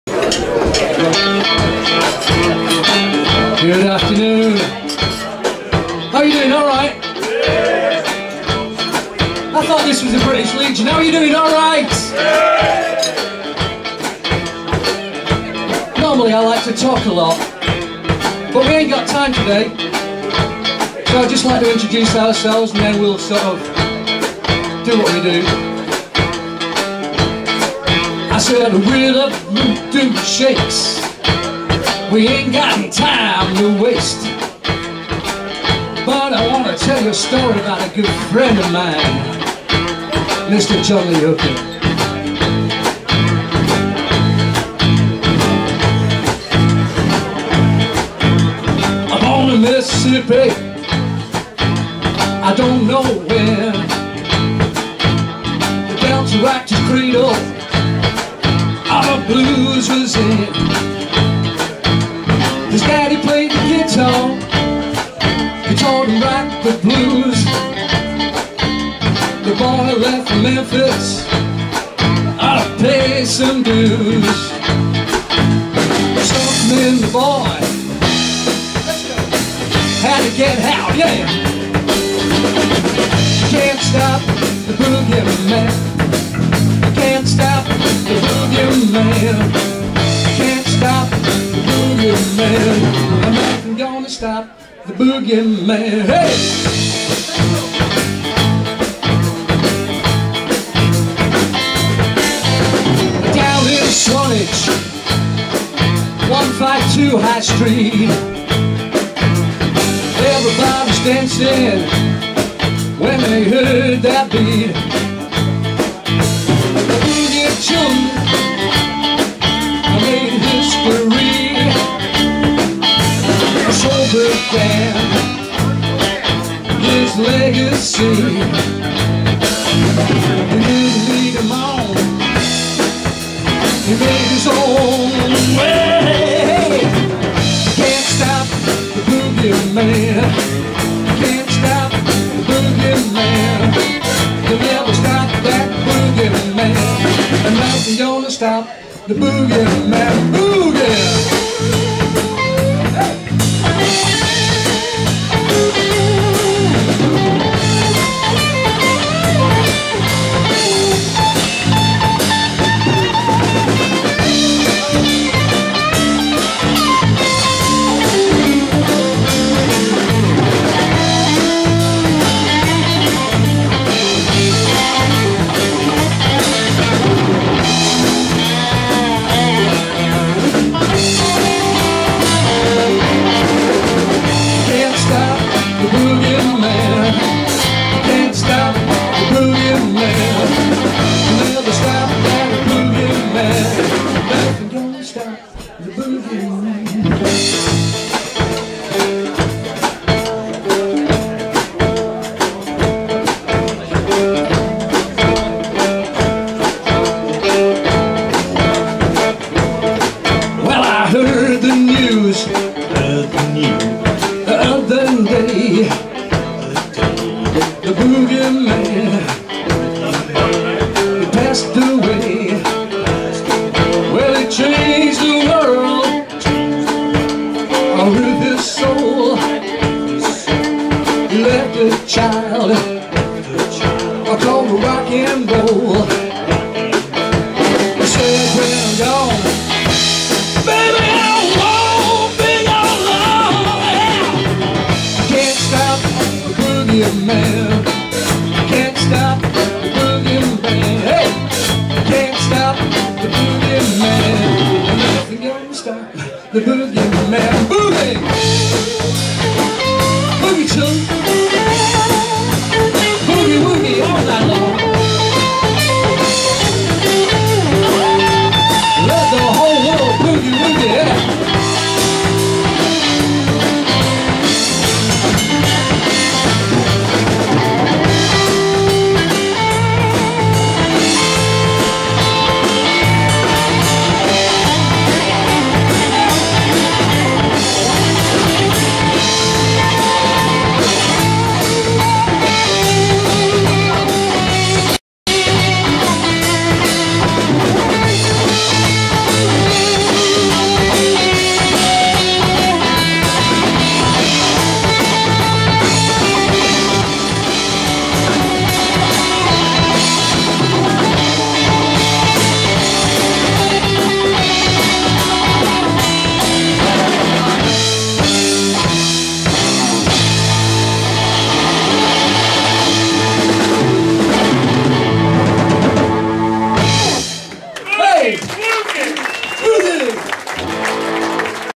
Stan's Blues Jamboree at Swanage Blues Festival - 051013 - The Voodoo Sheiks - JLHB
Stan's Blues Jamboree at Swanage Blues Festival - 051013 - The Voodoo Sheiks